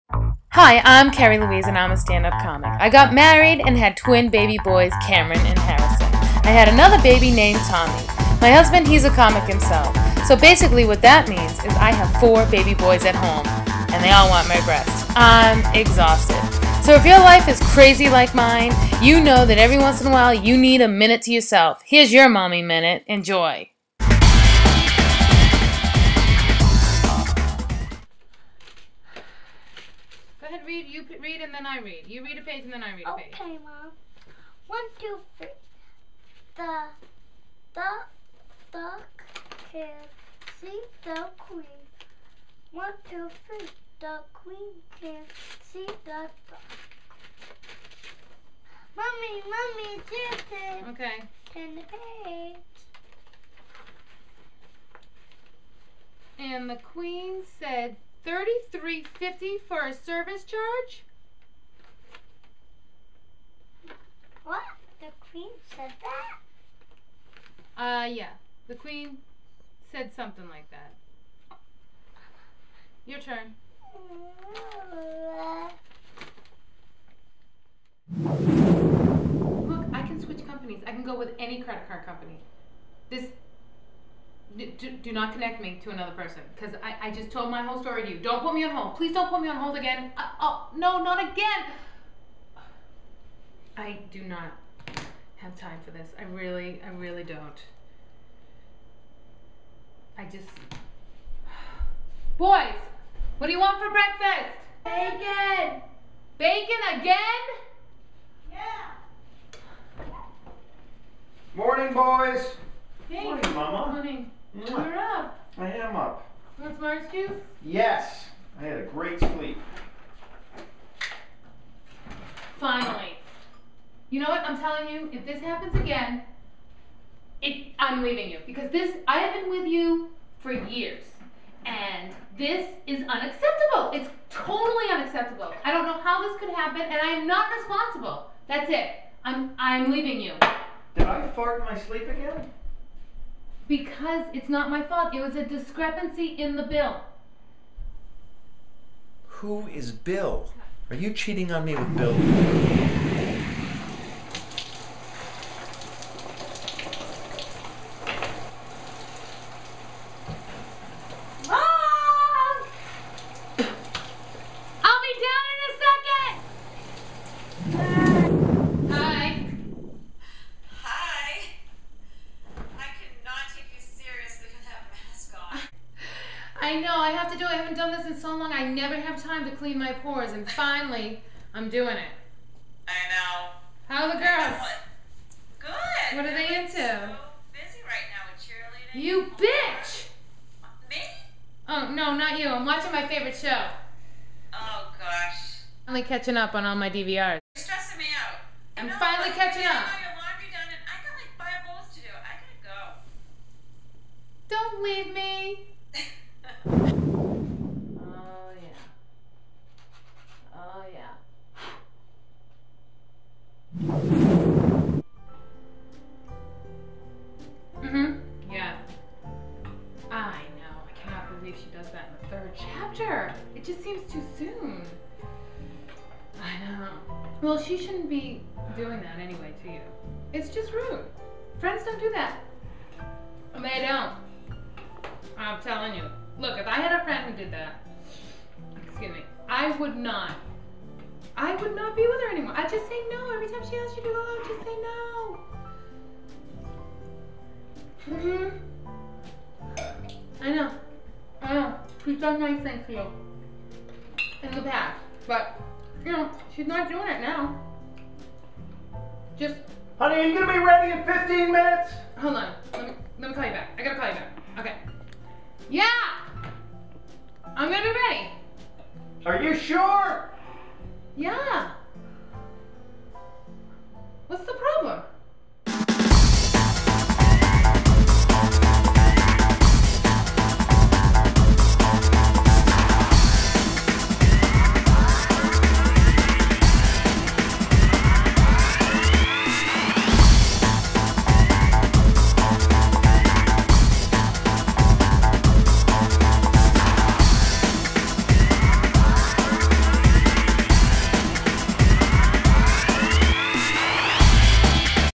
Multi Tasker
Mommy Minute shows how she gets it all done by MULTI-TASKING!!